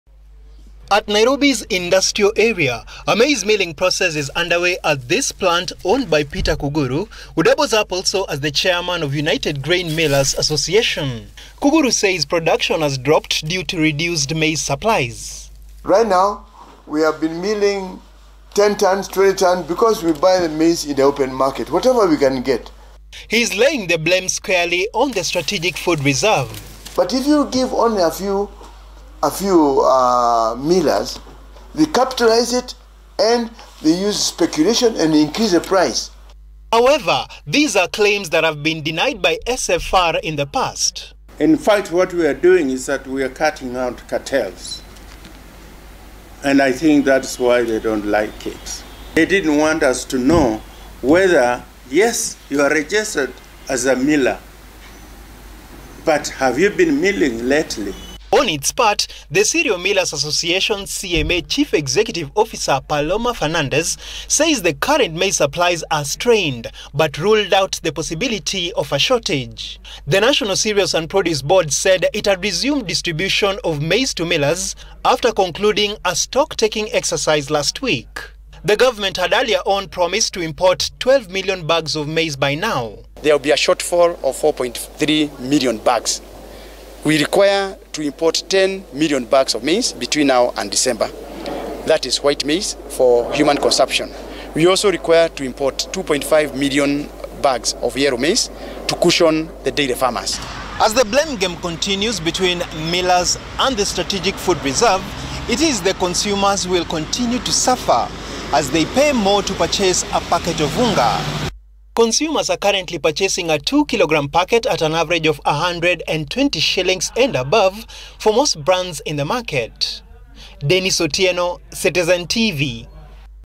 Millers running out of maize stocks
Small scale maize millers now want the government to sell maize directly to them or though the national Cereals and produce Board. The millers accuse the Strategic Food Reserve, SFR, of hoarding maize stocks leading to high flour prices. Denis Otieno reports.